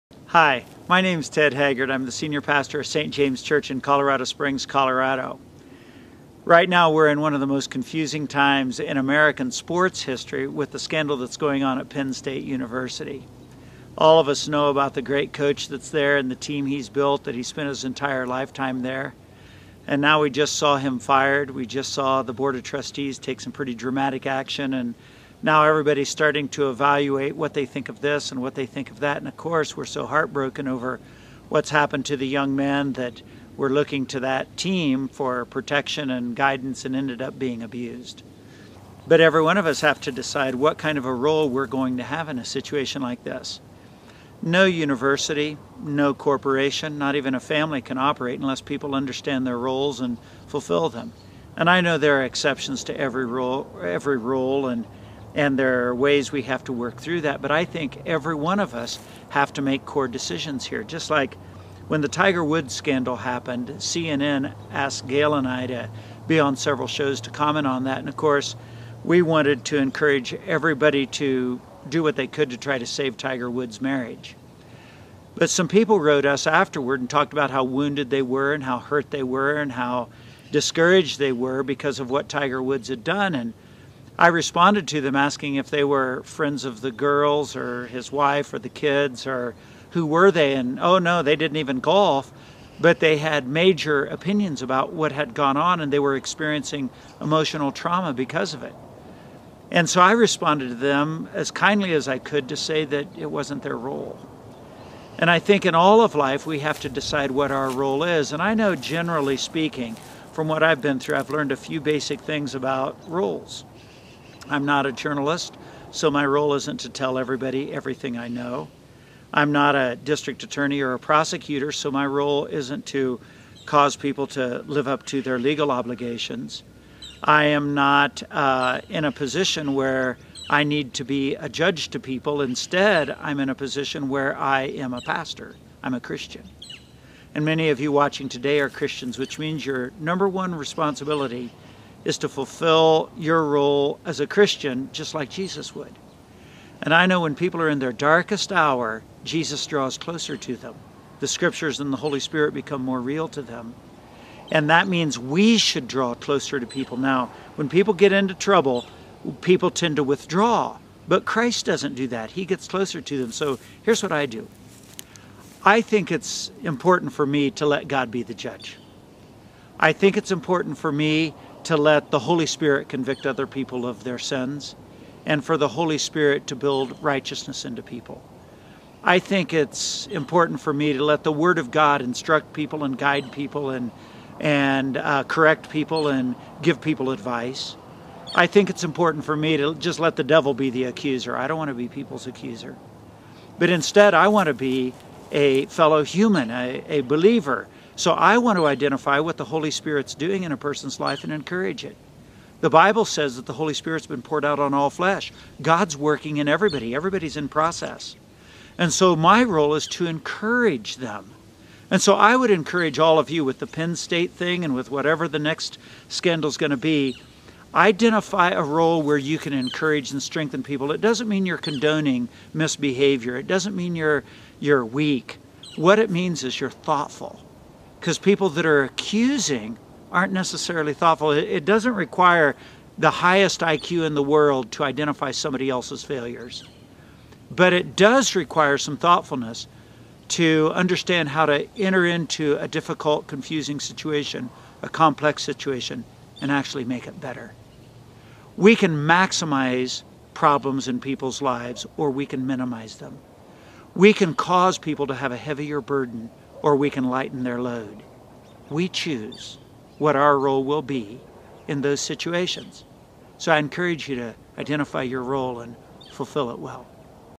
Ted Haggard comments on Penn. State crisis
Ted Haggard comments on Penn. State crisis thhp://tedhaggard.com Pastor Ted talks about what roles different people have in society. As Christians, what should our response be towards people, what role do we play?